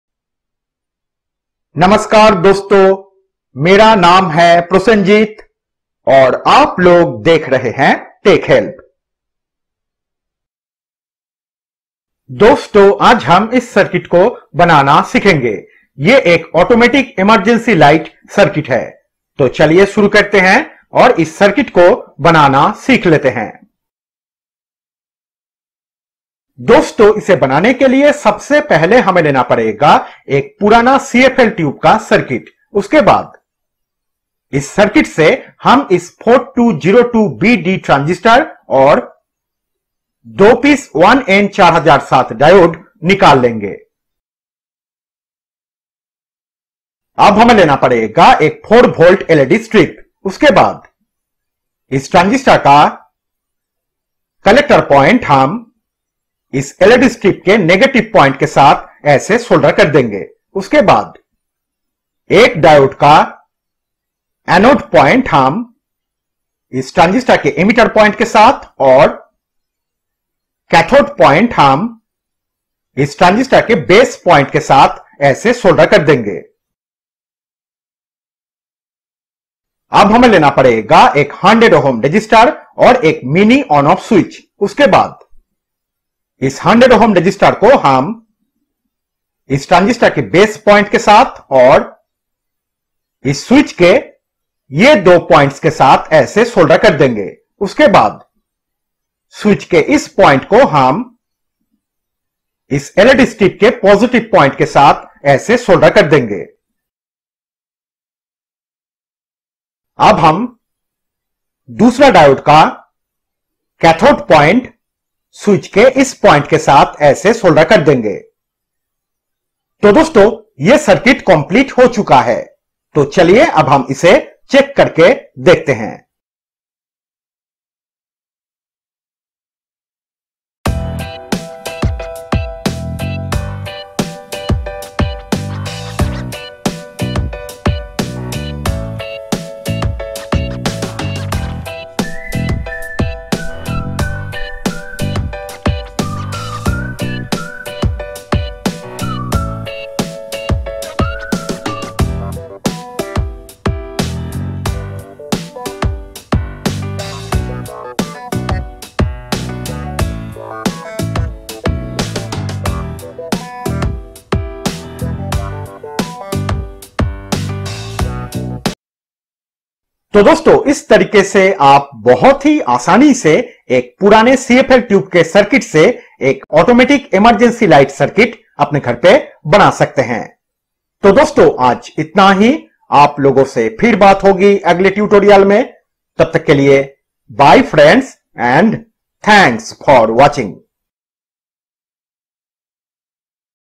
Make Automatic Emergency Light By An OLD CFL Tube Circuit..Diy Idea..Best Out Of Waste..
Friends in this video I will show you how to make automatic emergency light at home..Best out of waste..It is a easy electronics diy project which you can easily make at home..Friends we will use some waste materials like an old cfl tube circuit for making this automatic emergency light circuit..It is a fully automatic emergency light circuit which you can easily make at home..Friends it is a 3.7v to 4v emergency light..At first make the circuit then connect it with a 3.7v or a 4v battery..Friends in this video I will show you the complete process of making an automatic emergency light step by step..So friends if you want to know how to make an automatic emergency light by using waste materials at home then watch the video and follow my instruction..Thanks..